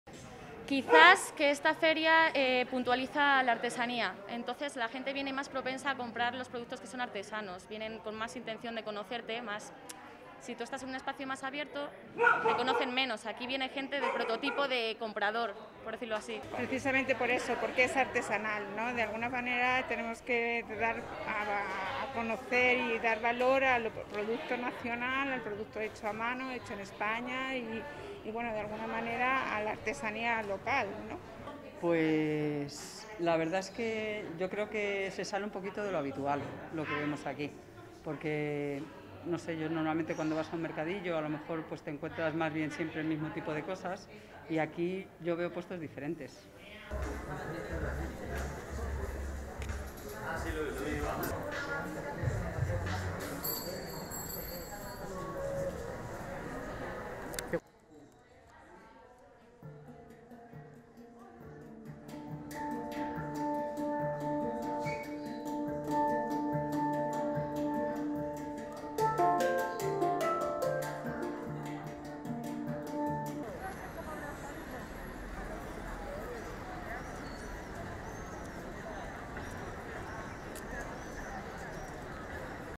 Artesanos de 'Madrid Craft Week' valoran la feria por su dedicación artesana
Las expositoras participantes en la feria artesana 'Madrid Craft Week', que se celebra en la capital hasta el 6 de junio, han reivindicado este mercado por poner el foco concretamente en los productos artesanos, lo que diferencia a esta feria, en su opinión, de otros mercados de la capital.